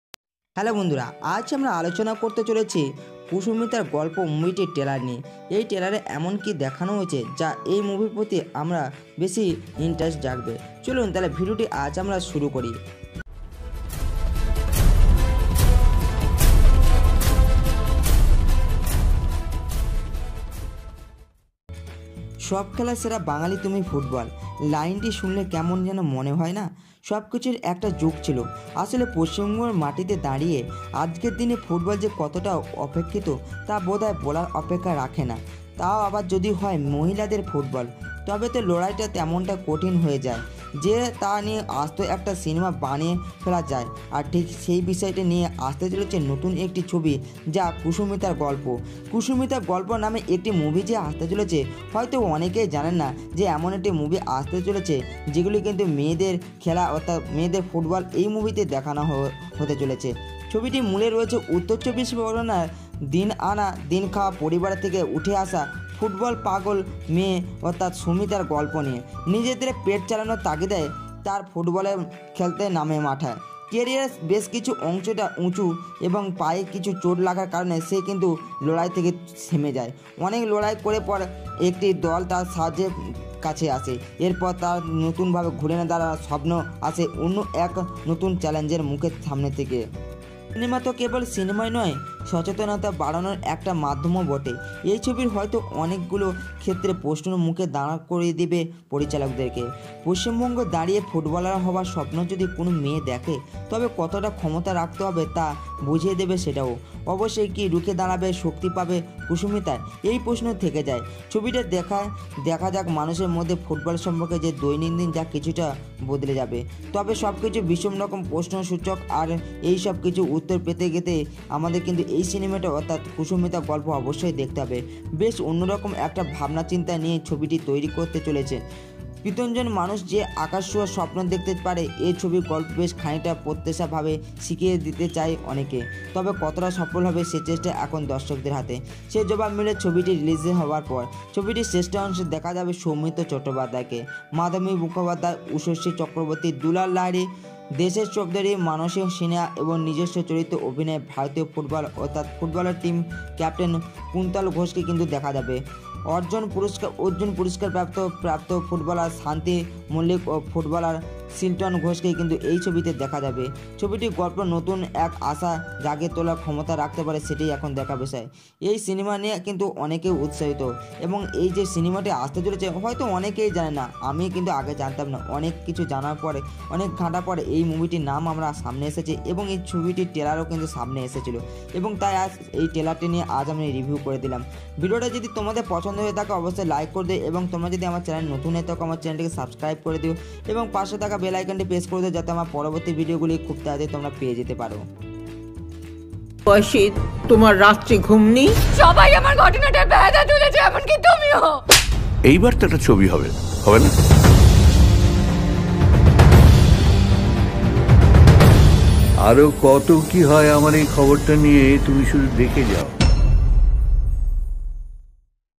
Kusumitar gappo : Trailer Review / Soumitra Chatterjee / Ushasie / Shilton / Kuntal
হ্যালো বন্ধুরা আজ আমরা টেলারিং করতে চলেছি কুসুমিতা গল্প মুভিটির কুসুম গল্প করে আজ আমরা বলতে চলেছি এই মুহুর্তের মধ্যে কী রয়েছে যা আমরা এই মুভিটি দেখার জন্য বাধ্যতামূলক হব